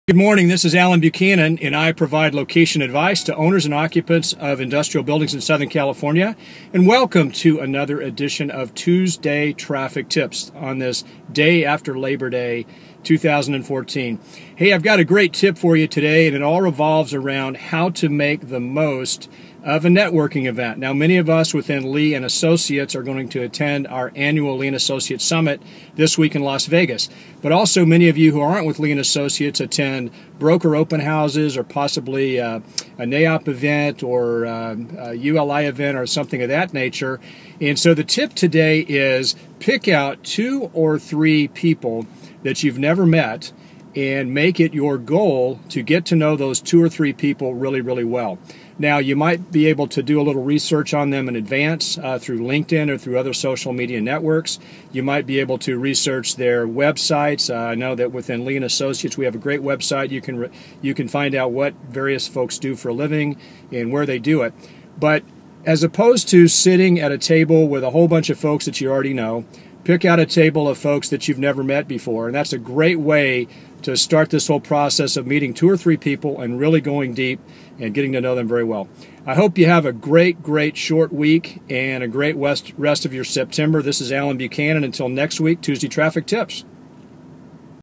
Maximizing a CRE event, Tuesday Traffic Tips
Maximizing a CRE event, Tuesday Traffic Tips. Have you ever attended a broker open house, NAIOP event or an annual meeting? Of course, we all have. But how do you make the most of this time away from everyday deal making? In this week's VIDEO tip, I discuss a foolproof way to maximize your commercial real estate event.